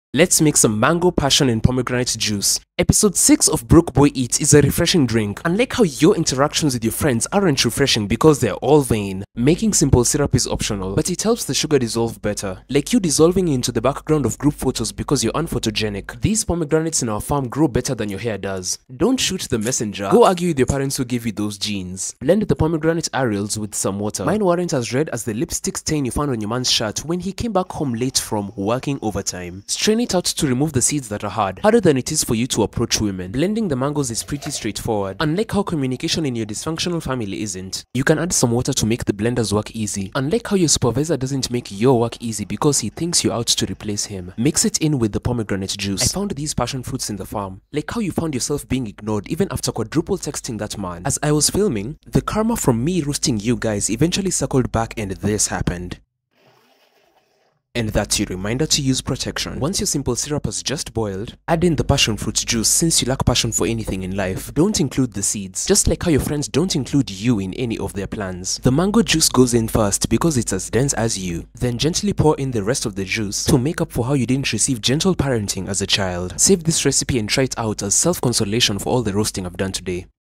Mango-Passion and Pomegranate Juice
This Mango-Passion and Pomegranate Juice is really refreshing for a hot summer day
ingredients;
2 mangoes
5 pomegranates
5 passion fruit
⅓ cup sugar
½ cup water